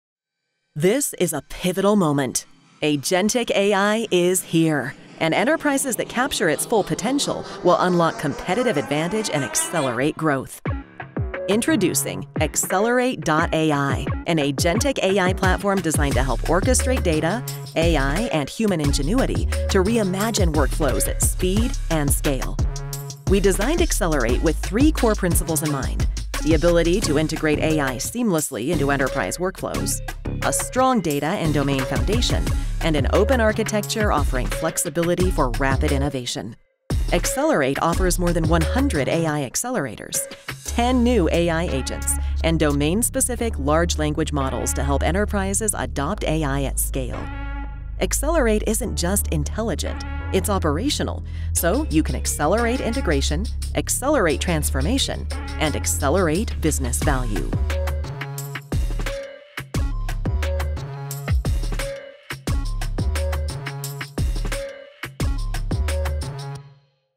EXLerate.AI: The Agentic AI Platform for Enterprise Transformation
EXL’s agentic AI platform helps enterprises reimagine workflows by seamlessly integrating EXL and third-party AI agents into business operations—driving efficiency, accuracy, scalability, and greater return on AI investment.